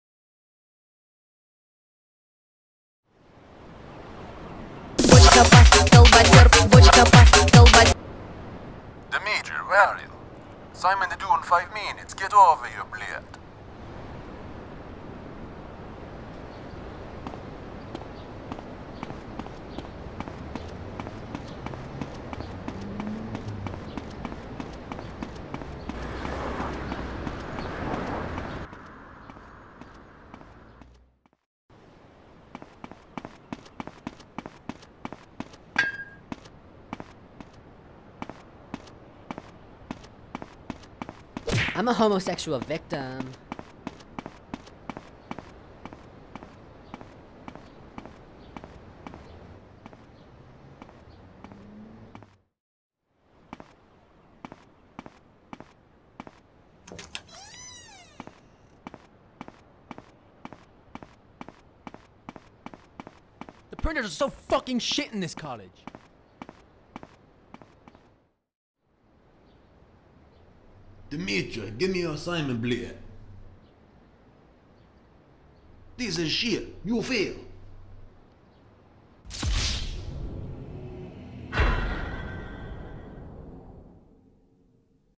TRANSIT: A GTA Inspired Short Film
A GTA inspired short film telling a story about Dimitri who gets a phone call from his tutor Roman, prompting him to arrive at college to submit his assignment on time. Follow his journey to see what happens next.

Shot by: Rolands Zilvinskis, Oisin Ralph
Edited by: Rolands Zilvinskis
VFX: Rolands Zilvinskis, Oisin Ralph
Actors: Jack Horgan, Oisin Ralph, Rolands Zilvinskis